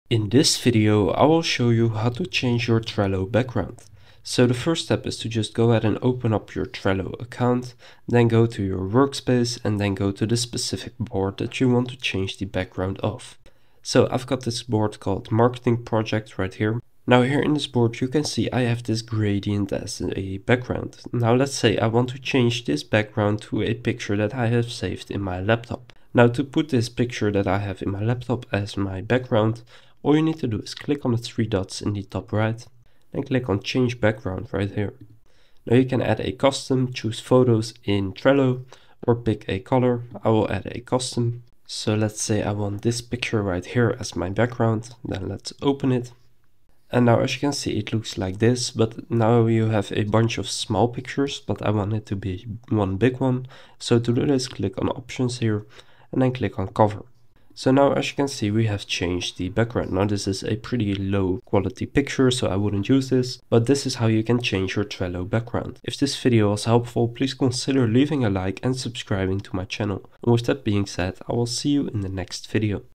How to Change Trello Background (2025)
In this video I'll show you how to change Trello background
By following the steps in this video you'll be able to change the background of your board in Trello, Trello is a popular project management tool which can be used for different purposes such as game development or as a sales crm.
Contents:
- how to change trello background
- trello change background image
- how to change trello board background